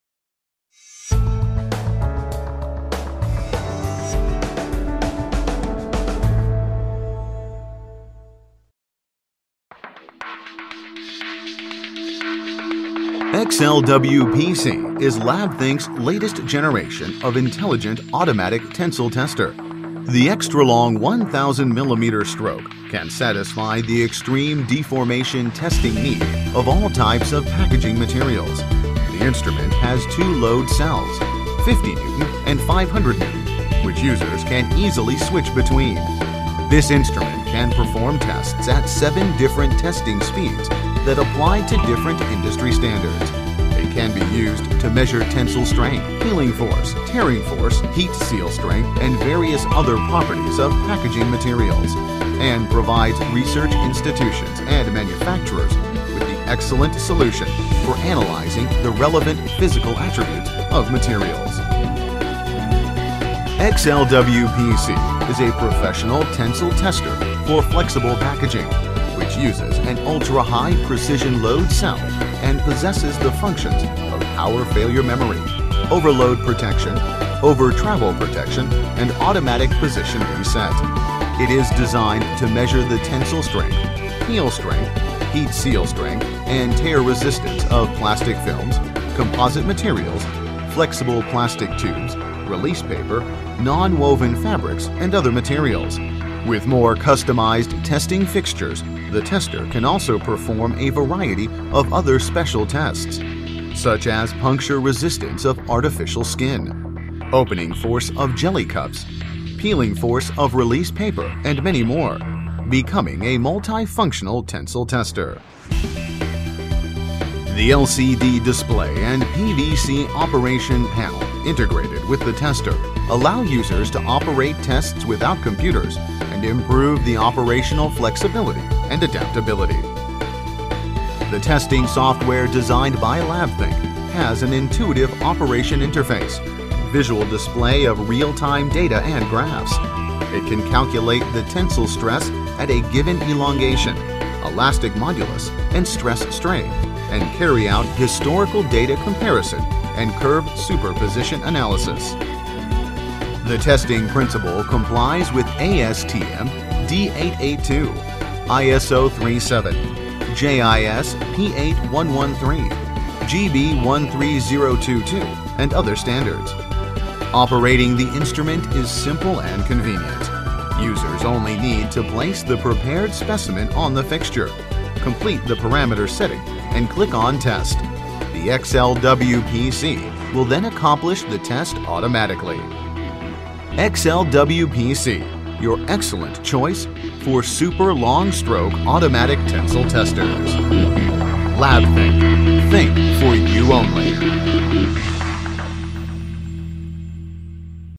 XLW (PC) Tensile Strength Tester - Labthink®
XLW (PC) Tensile Strength Tester is professionally applicable to the determination of force properties of various flexible packaging materials, with high precision (better than 0.5% of reading accuracy) and multiple test ranges. The instrument is designed with 7 independent test modes and could perform tests at seven different test speeds to meet various test conditions.